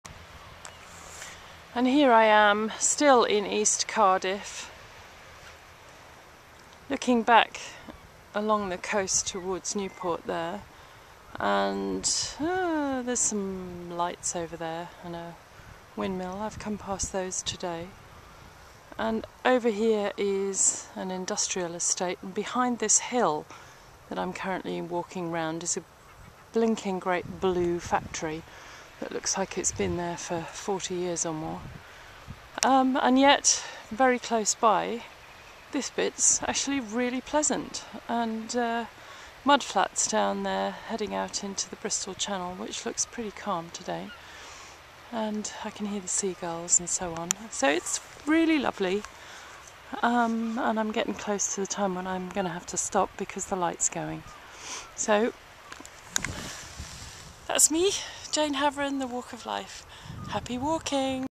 Day 6 Walking the Wales Coast Path, East Moors, Cardiff. 2013-06-01
A little hillock in front of the steel works at East Moors - eastern end of Cardiff